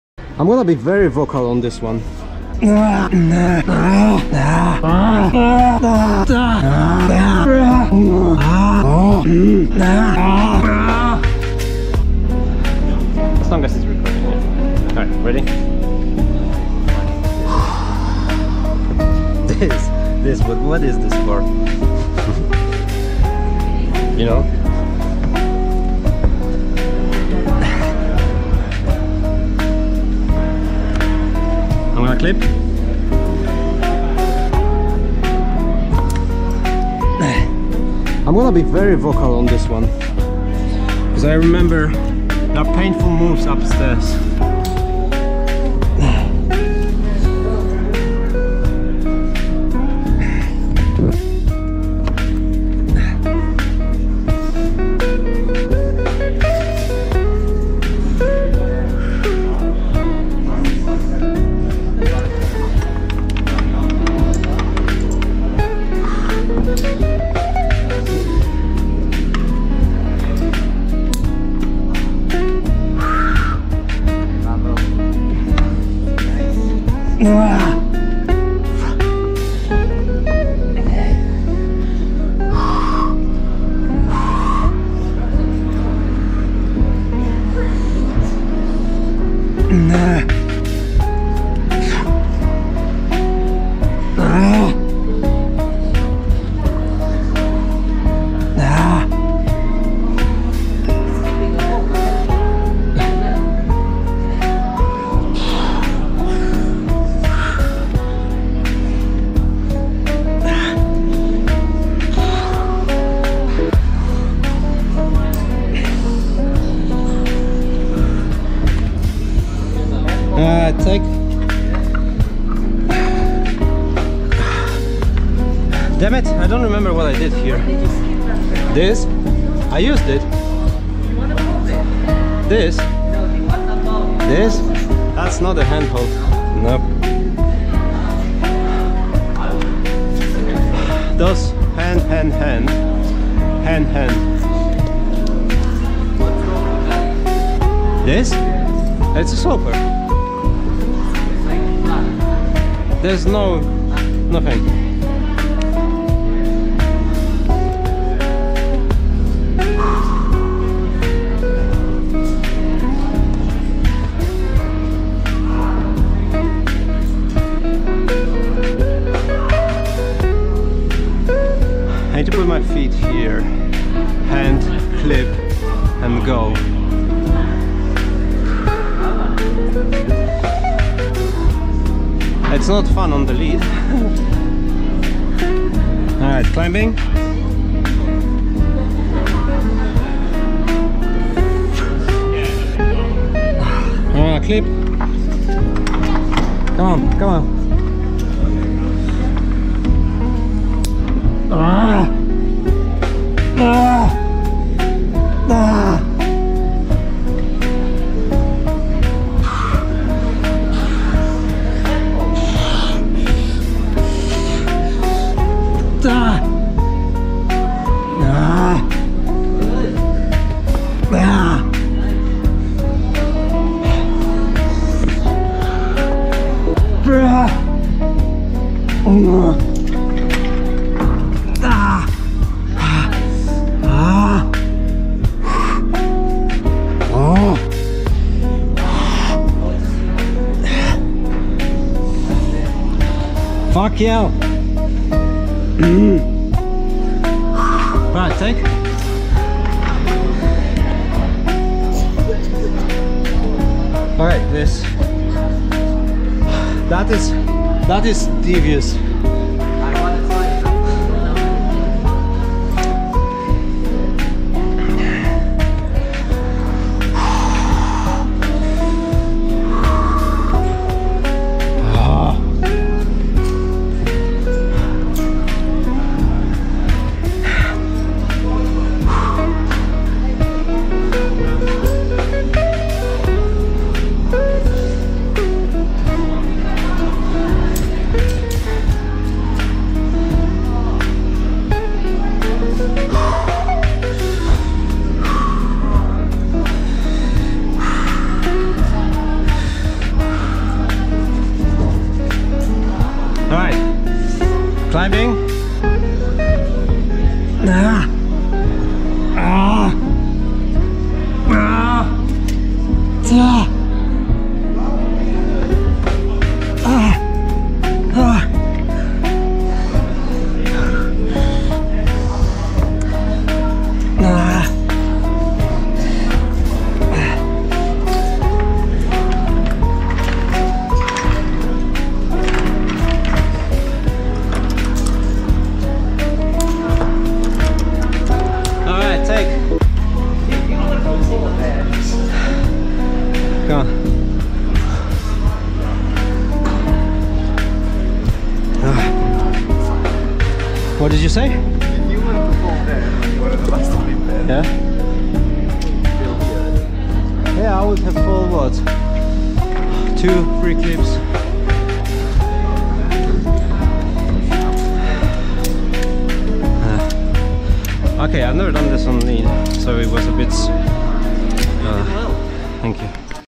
POV Lead 6C+ while being very vocal
Sometimes,  or loads of time I am being very vocal when climbing. But these are all power screams that help me stay at the wall and keep going!

Recording:
256GB SD card